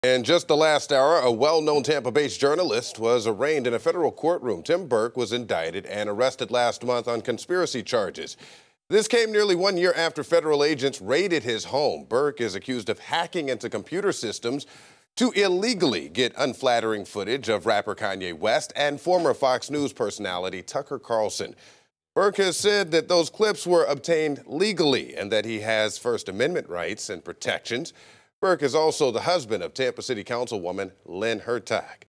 Tampa journalist arraigned over alleged hack & leak of Tucker Carlson, Kanye West interview
Tim Burke has claimed the clips were obtained legally and that he had a First Amendment right to distribute the material.